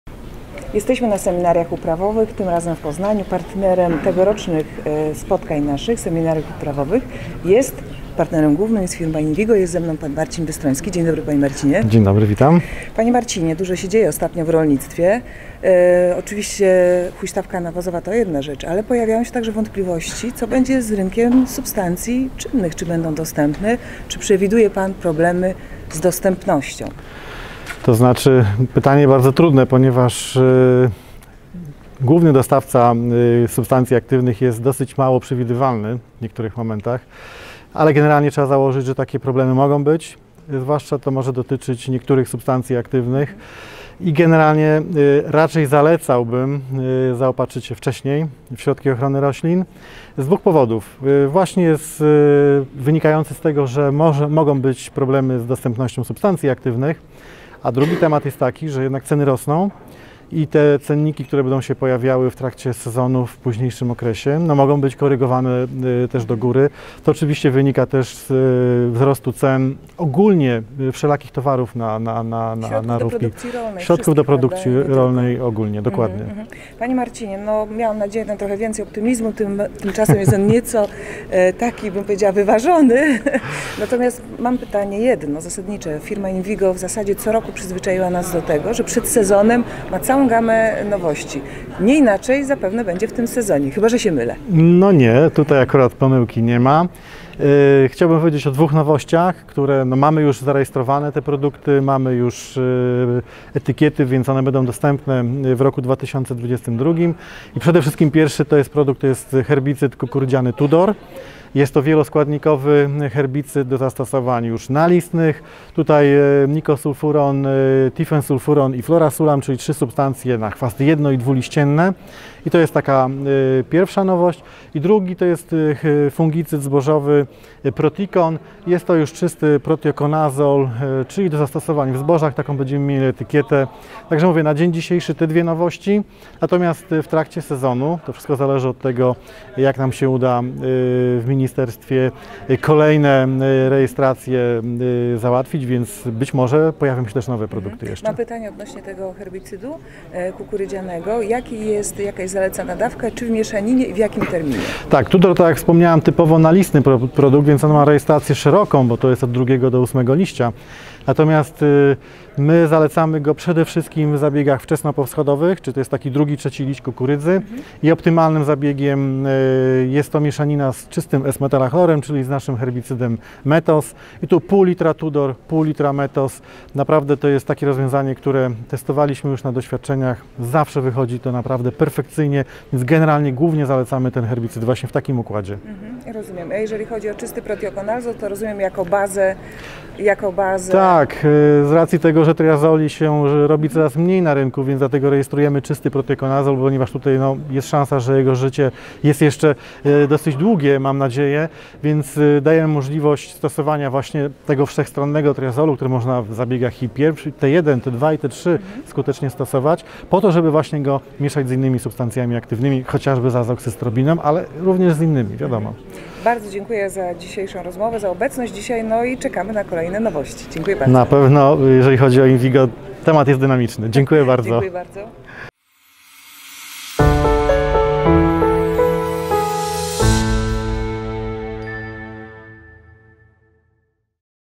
Co z dostępnością substancji czynnych na rynku? Marcin Bystroński (Innvigo)
Jednym z prelegentów podczas naszych seminariów uprawowych był Marcin Bystroński z firmy Innvigo, który w mówił o innowacjach w uprawie kukurydzy.

Jaka jest przyszłość rynku substancji czynnych i czy specjalista przewiduje problemy z ich dostępnością? Co z cenami środków do produkcji?

Jak co roku firma Innvigo przedstawia gamę nowości przed sezonem wiosennym. Czego możemy spodziewać się w 2022 roku?

O to w rozmowie z Marcinem Bystrońskim pyta dr Maria Walerowska, zastępca redaktora naczelnego top agrar Polska.

Jak się zalogować na swoje konto? ↩️

Jeśli jeszcze nie masz prenumeraty sprawdź aktualną ofertę i wybierz pakiet dla siebie ↩️